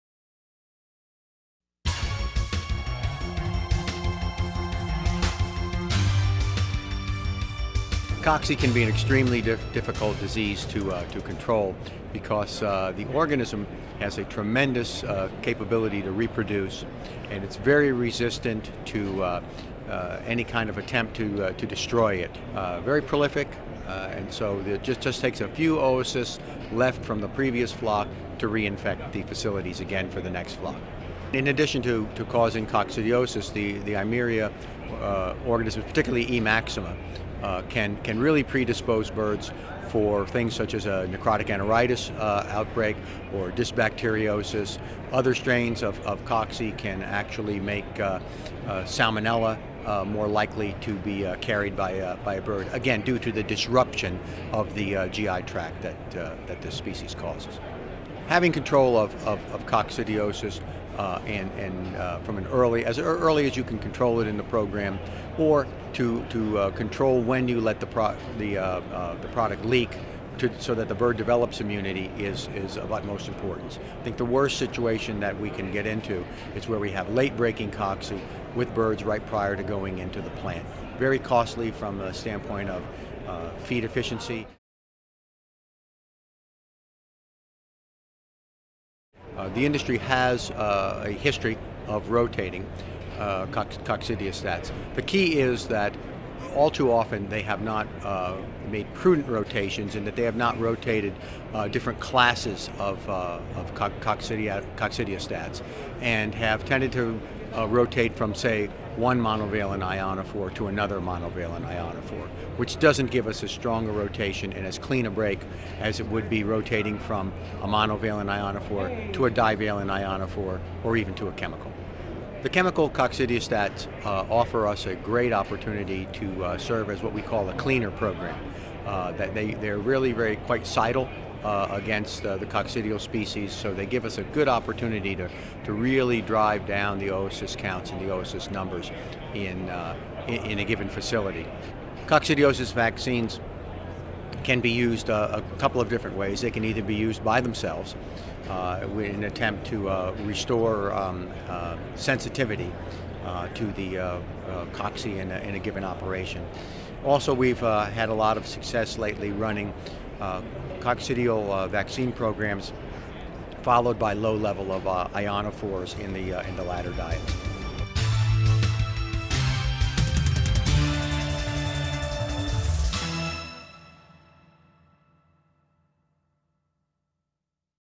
Poultry Health Today - Coccidiosis: Why is it still so difficult to control?
Coccidiosis can be extremely difficult to manage in poultry because the organism that causes it "has a tremendous capability to reproduce and it's very resistant to any kind of attempt to destroy it," says Mark LaVorgna, PhD, nutritionist and technical services director for Zoetis Inc. He recommends developing a long-term coccidiosis-management plan that employs all available tools.